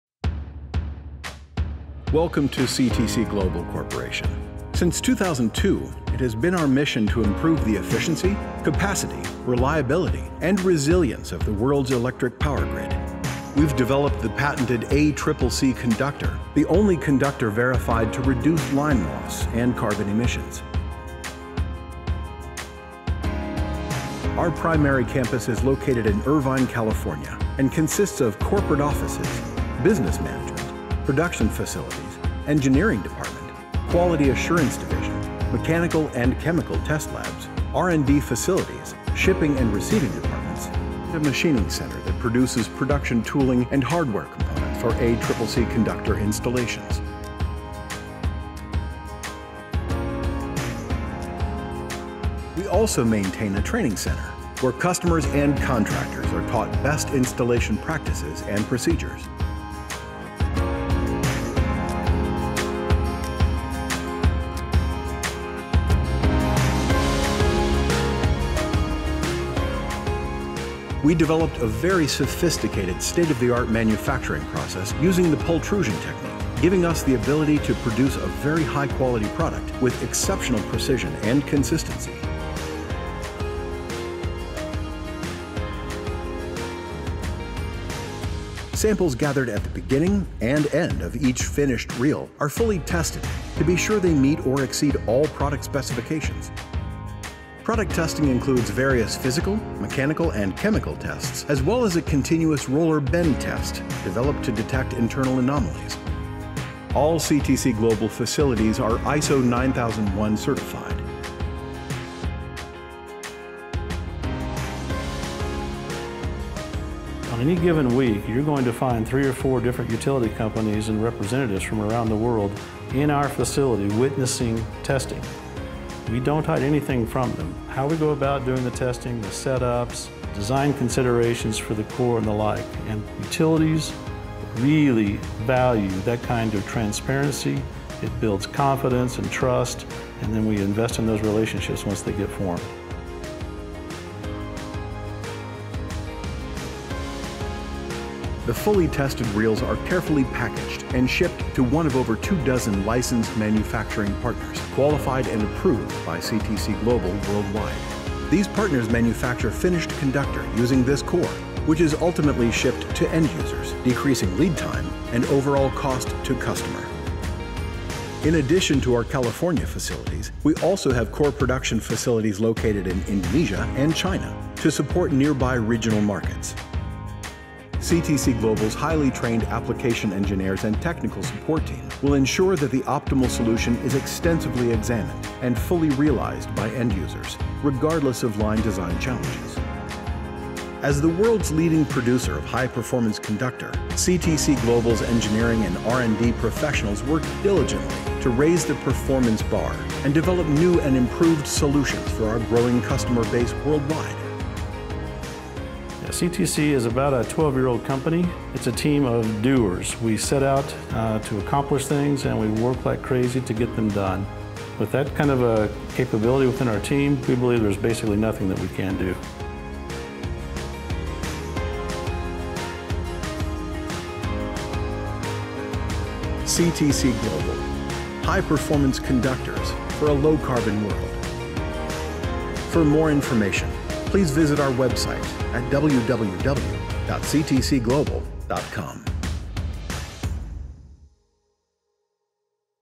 Welcome to CTC Global
CTC Global developed, tested and produces the composite core used in ACCC® Conductors. Produced in association with over 35 global stranding partners, ACCC® Conductors leverage advanced carbon fiber technology to help improve the efficiency, capacity, reliability, and resiliency of electric power grids worldwide. Over 165,000 km of ACCC® Conductor has been deployed to more than 1,250 projects in 65 countries.